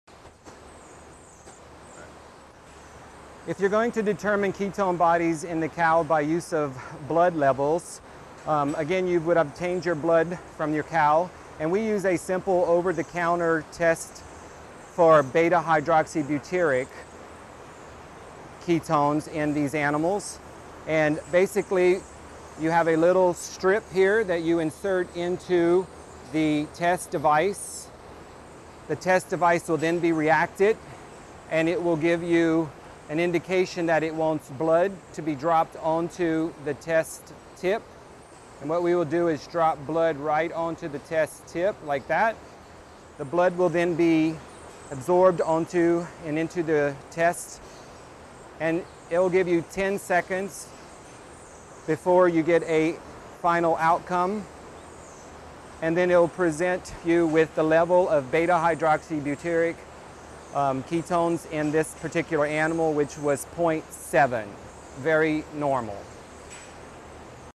Performing a Ketone Bodies Test Using a Blood Sample
Dr. Billy Smith explains how to do a ketone bodies test using a blood sample for veterinary students.